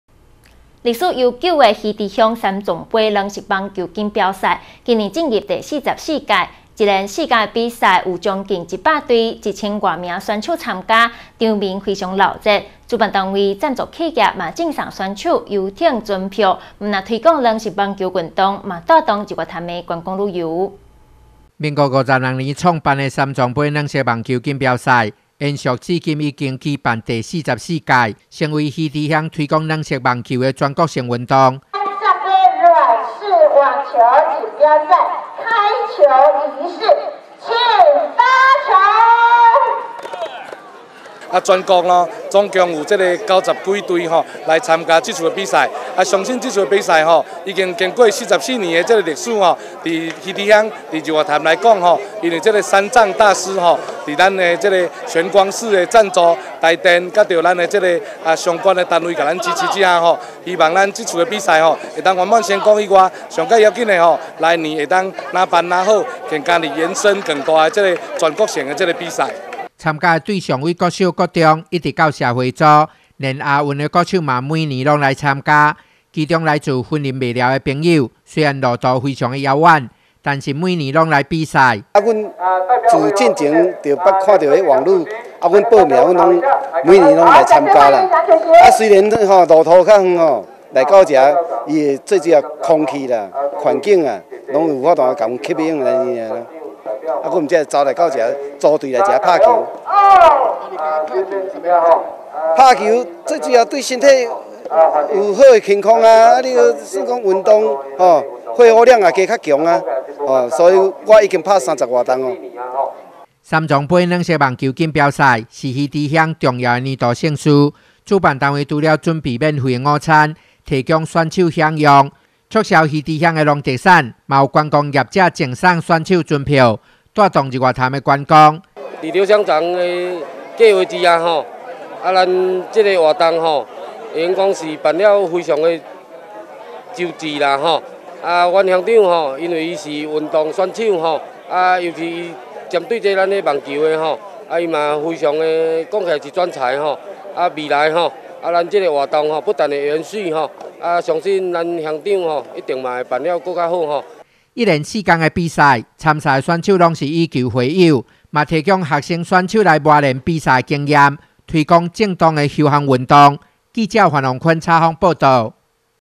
南投新聞 魚池第44屆三藏盃軟網錦標賽
歷史悠久的魚池鄉三藏盃軟網錦標賽，今年邁入第44屆，一連四天的比賽，近百個隊伍，一千多名選手參加，場面熱鬧，大會準備免費午餐提供選手享用之外，主辦單位、贊助企業也贈送選手遊艇船票，不僅推廣軟式網球運動，也帶動日月潭觀光旅遊。